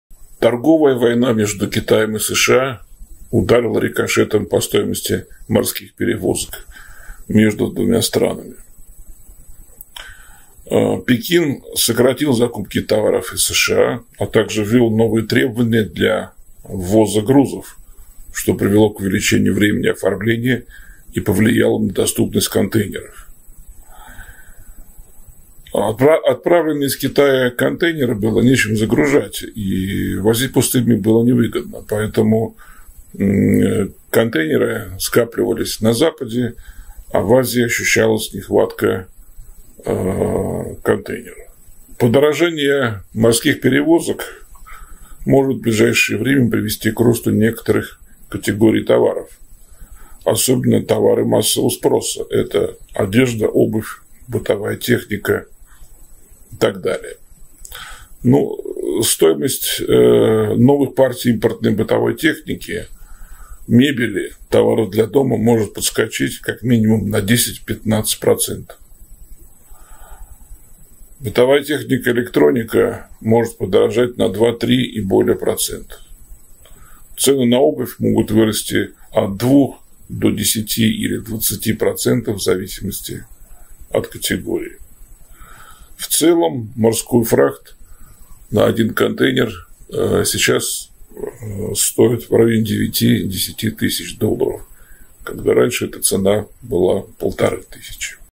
Россиян предупредили о подорожании техники и одежды
В ближайшем будущем в России подорожают некоторые категории импортных товаров — одежда, обувь и бытовая техника. Об этом сообщает «РБК» со ссылкой на участников рынка. Подробнее о ситуации в беседе с «Городовым» рассказал квалифицированный инвестор Владимир Трегубов.
Он пояснил, что такие перемены — следствие торговой войны между Китаем и США, которая рикошетом ударила по стоимости морских перевозок между двумя странами. Пекин сократил закупки товаров из США и ввел новые требования для ввоза грузов, что повлияло на доступ контейнеров и увеличило время оформления товаров. Подорожание морских перевозок, в свою очередь, ударит по ценам на вышеуказанные товары.
«В целом морской фрахт на один контейнер сейчас стоит в районе 9-10 тысяч долларов, когда раньше стоил полторы тысячи», — пояснил Трегубов.
Так, стоимость новых партий импортной бытовой техники, мебели и товаров для дома, по его прогнозм, подскочит как минимум на 10-15 %. Бытовая техника и электроника поднимется в цене на 2-3 % и более. Обувь подорожает от 2 до 10-20 % — это будет зависеть от ее категории.